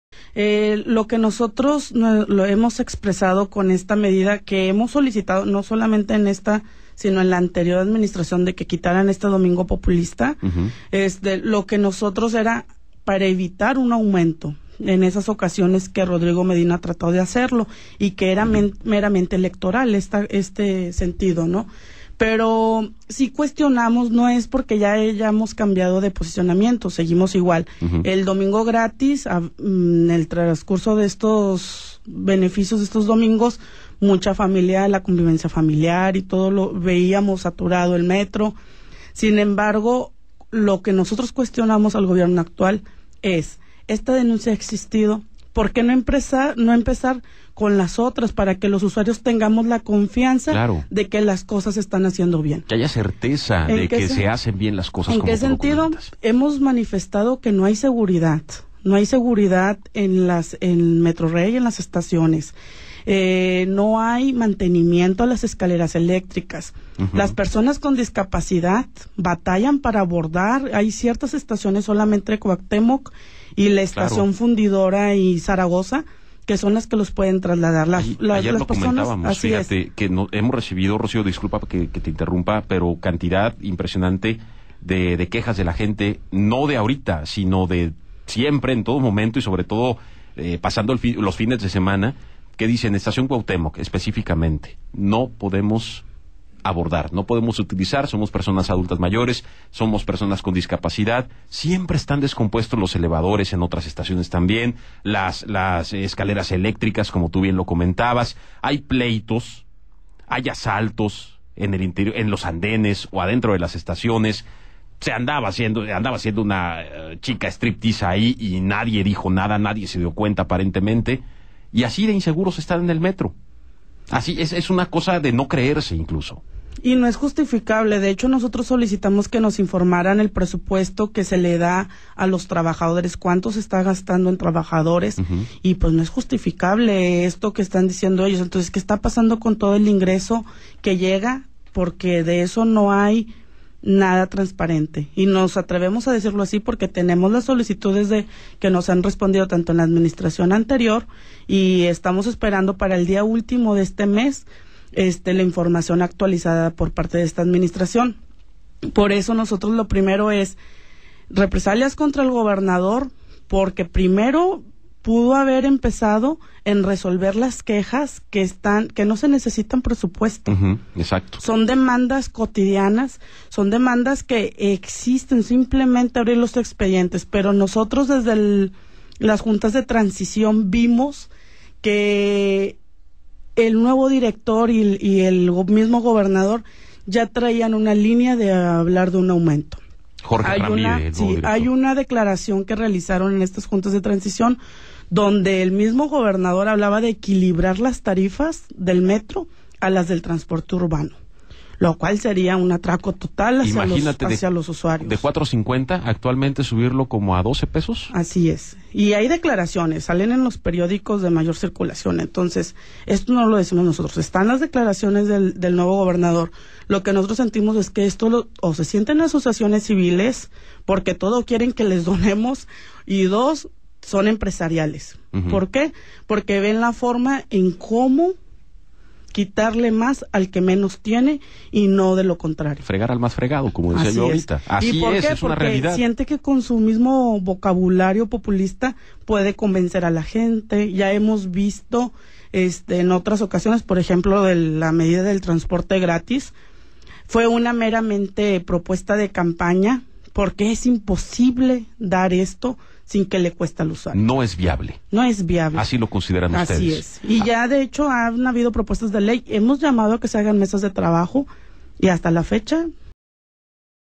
Unete Pueble en contra de cobrar el Metro los domingos
Integrantes de Unete Pueblo se manifiestan en contra de eliminar los domingos gratis del Metro.